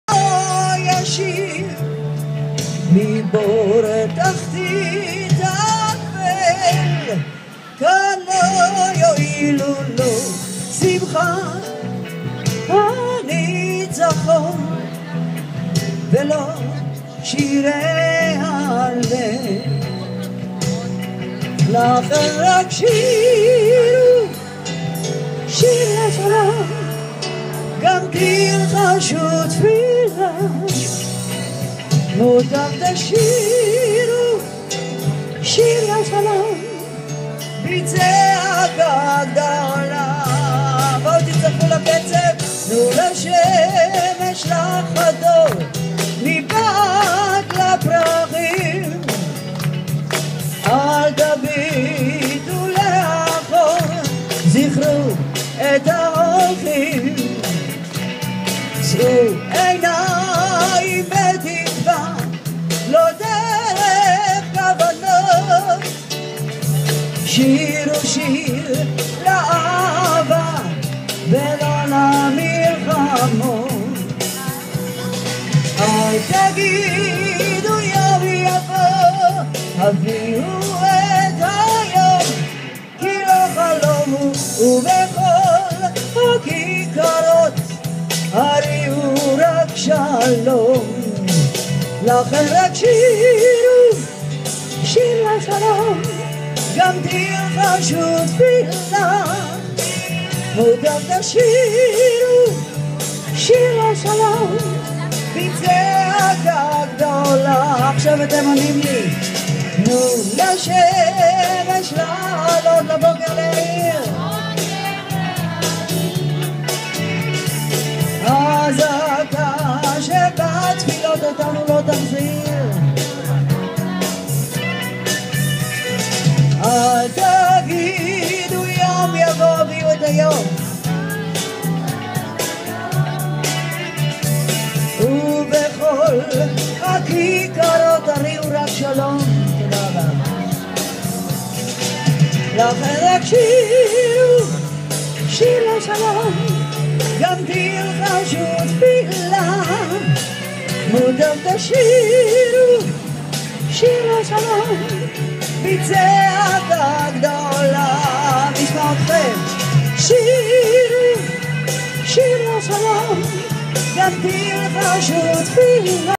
Miri Aloni
Performs at Nahalat Binyamin in Tel Aviv.